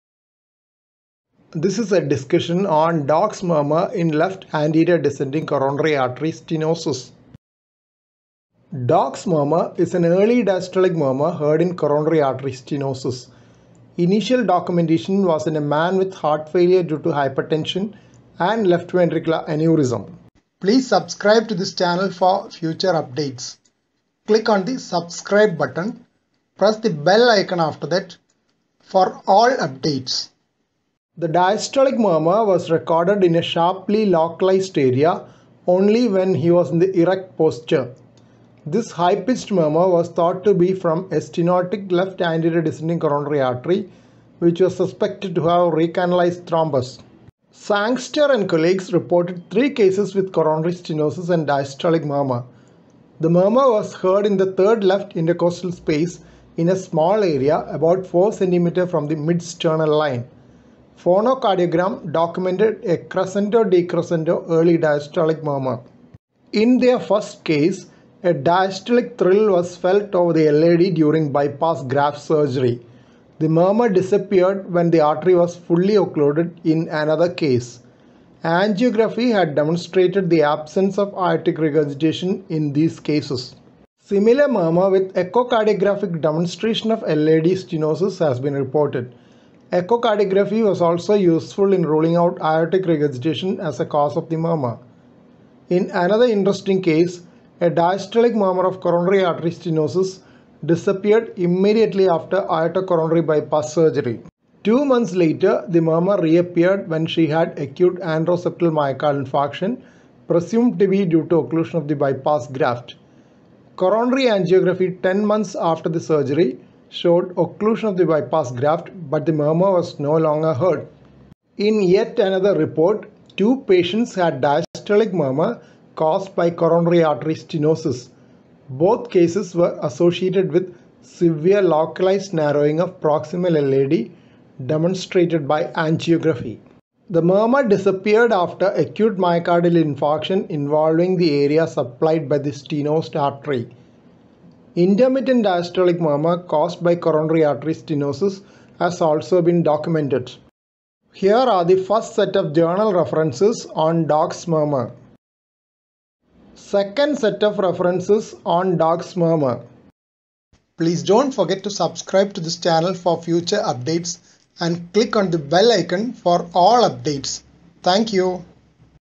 Dock’s Murmur in LAD Stenosis
Discussion on Dock’s murmur in left anterior descending coronary artery stenosis.
Dock’s murmur is an early diastolic murmur heard in coronary artery stenosis. Initial documentation was in a man with heart failure due to hypertension and left ventricular aneurysm.